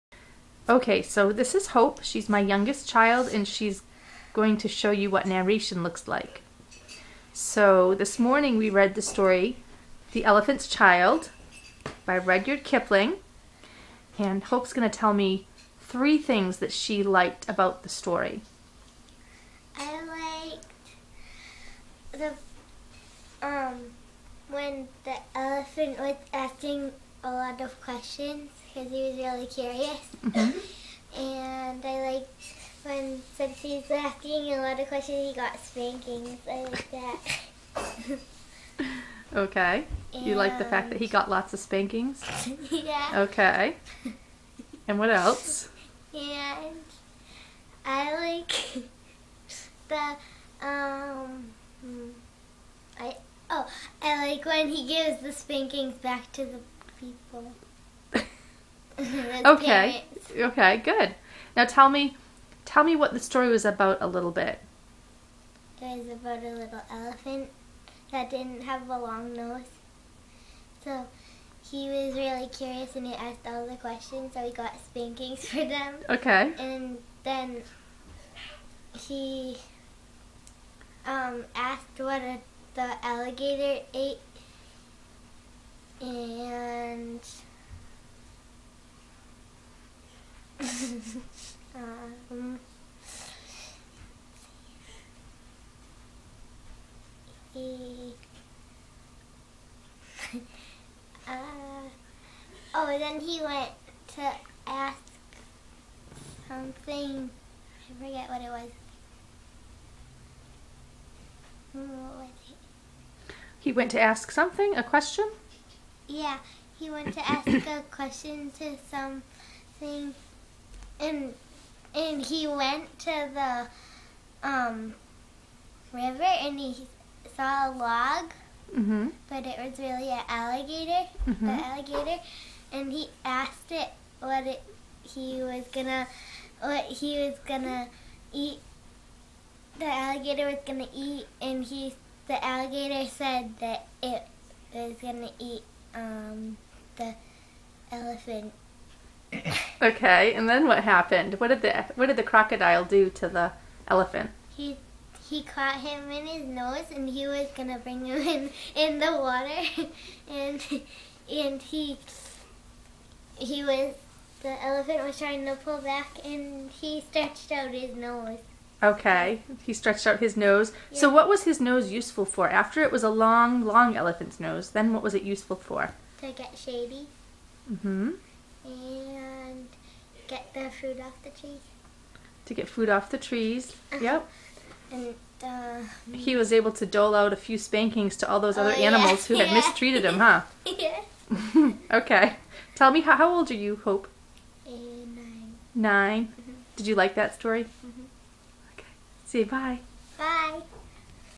Hope's narration
Retelling The Elephants Child by Rudyard Kipling